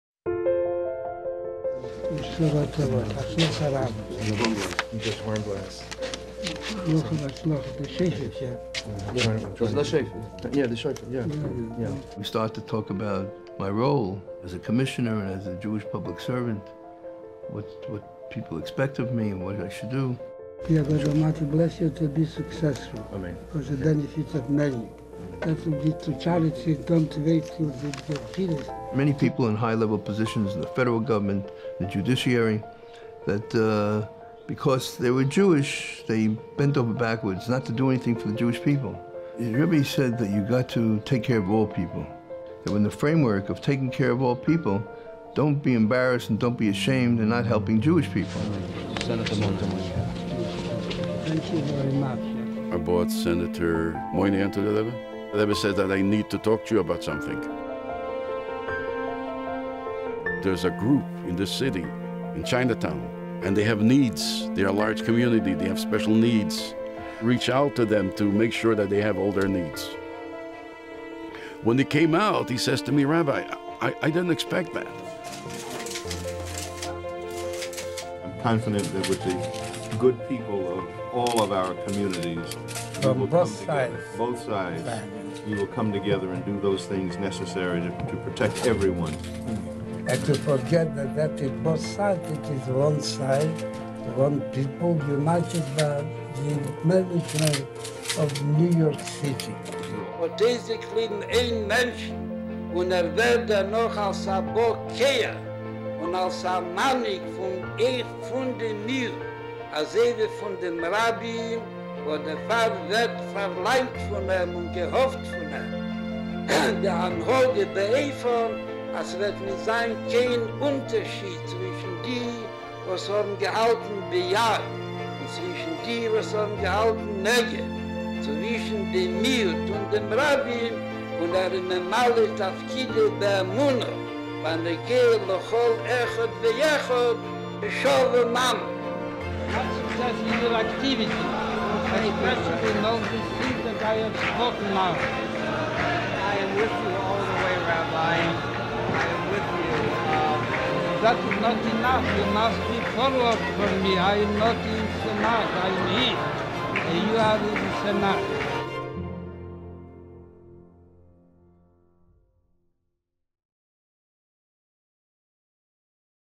Out of Many, One | The Lubavitcher Rebbe on the American Elections
As citizens of the United States head out to vote, we share a presentation of the Rebbe's talks and statements regarding the American presidential election.

Somewhat unusually for a Chassidic leader, the Rebbe would publicly address the current events of his time during his Farbrengens. He also warmly received many politicians who came seeking his counsel and blessing.

While he touched on a wide range of issues related to the United States, the Rebbe would often return to a fundamental principle of it's founding: the importance of uniting the entire nation around a common goal. Or as the country's motto declares, "E pluribus unum - Out of many, one."